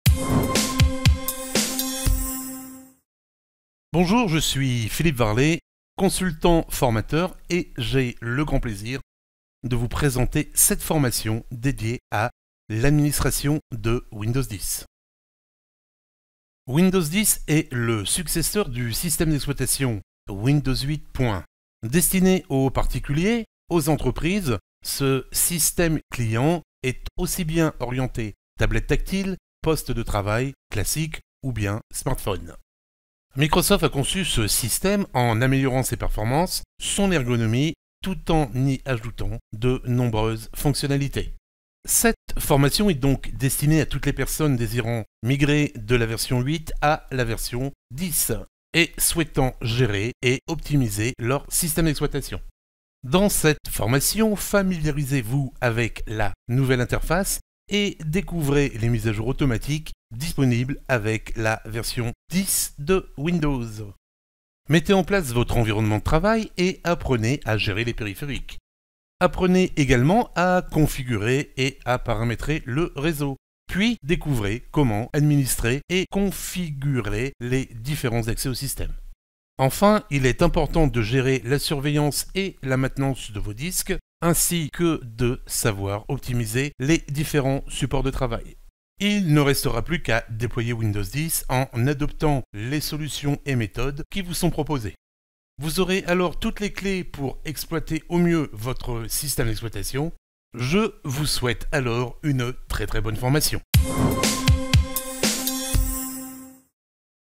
Windows 10 : L'administration - trailer | video2brain.com
​Mettez en œuvre la configuration, l'administration, et le déploiement de Windows 10

Optimisez les performances de votre système d'exploitation en adoptant Windows 10 ! Si vous êtes déjà utilisateur de Microsoft Windows ou que vous souhaitez améliorer votre environnement de travail, découvrez cette formation de Philippe Varlet. Prenez en main la nouvelle ergonomie et les nombreuses fonctionnalités adaptées à la tablette tactile, au poste de travail classique ou encore au smartphone. Familiarisez-vous avec l'interface et découvrez les mises à jour automatiques disponibles avec la version 10 de Windows. Mettez en place votre environnement de travail, apprenez à gérer les périphériques et à configurer les réseaux. Puis, découvrez comment paramétrer les différents accès. Enfin, gérez la surveillance et la maintenance de vos disques et déployez votre système à travers les différentes solutions offertes. Vous serez alors en mesure d'administrer Windows 10 selon vos besoins et desiderata.